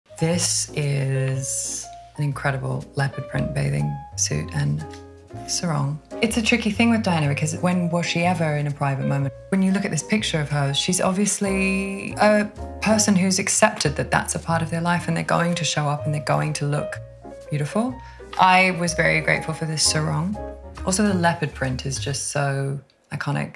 The Crown's Elizabeth Debicki On Princess Diana's Leopard Print Bathing Suit
The Crown's Elizabeth Debicki revisits one of Princess Diana's most iconic looks.